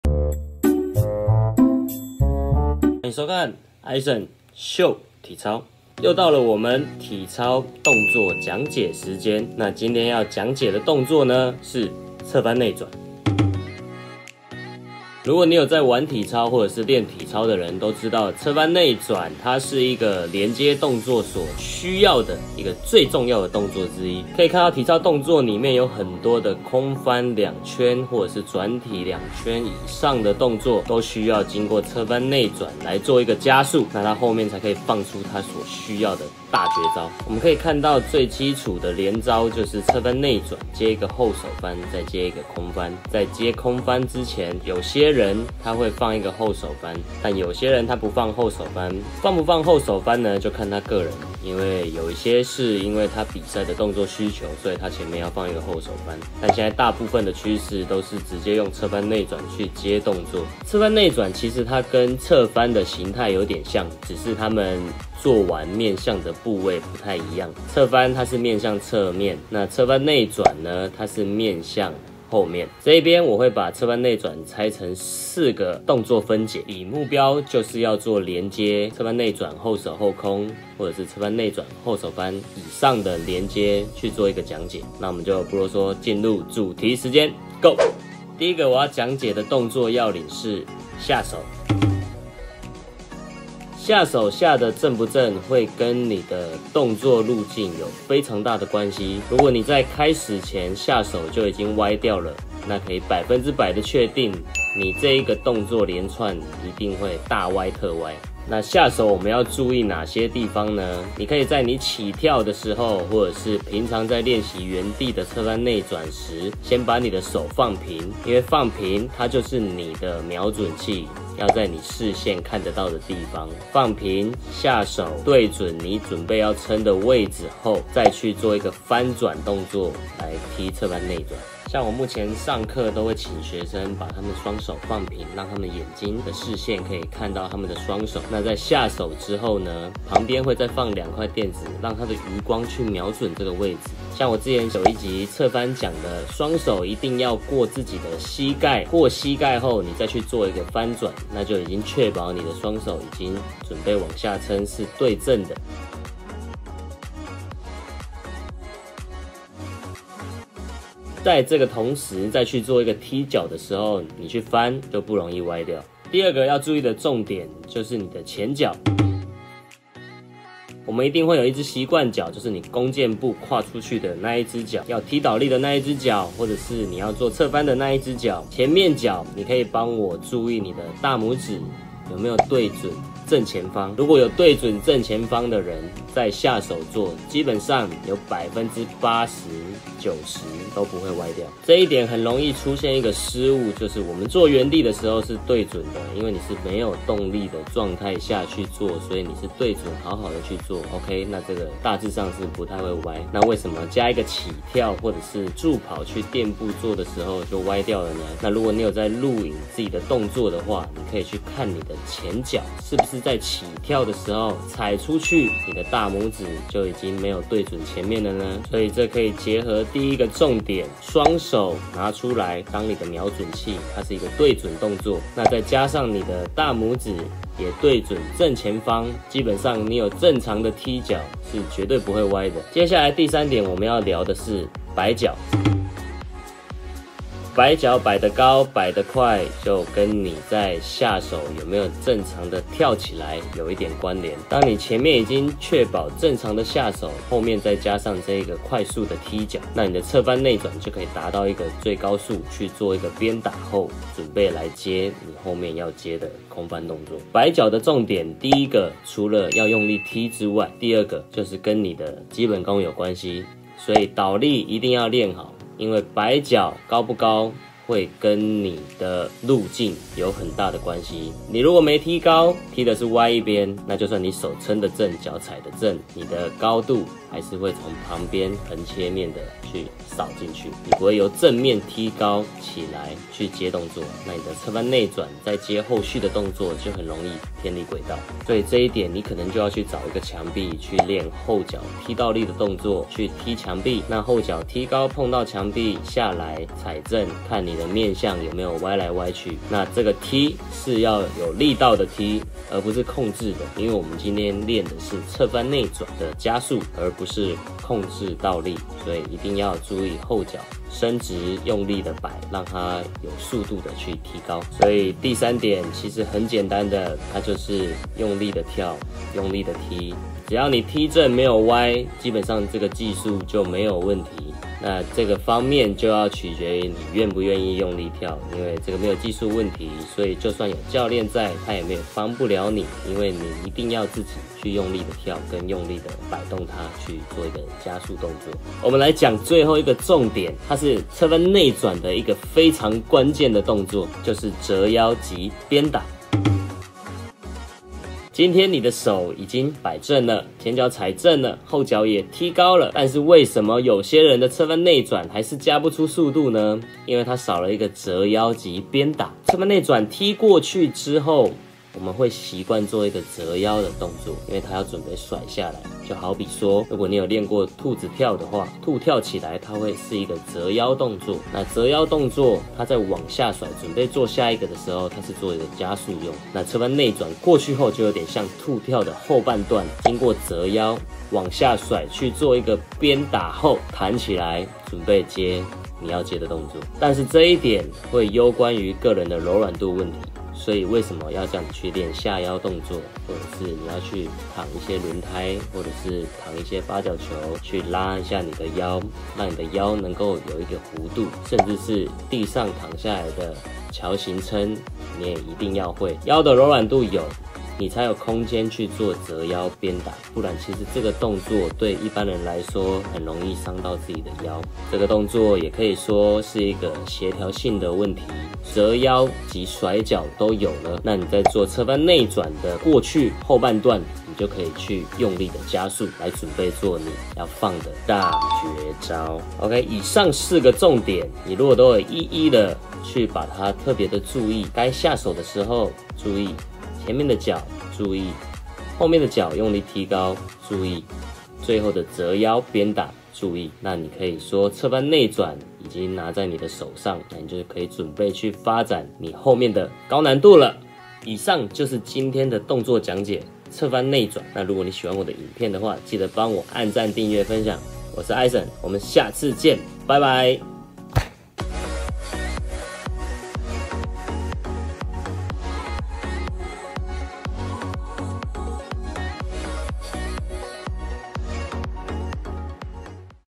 【側翻內轉】體操｜如何才能翻的正翻的穩翻的有動力｜大空翻最需要的動力
當你想要放一個大空翻最需要的動作之一
-側翻內轉-
沒有這個加速動作不會有其它的動作變化
如何翻的正翻的穩翻的有動力
看下去丟災啊！！

🤸🏻‍♂️追蹤我的IG►►coacheisen🤸🏻‍♀️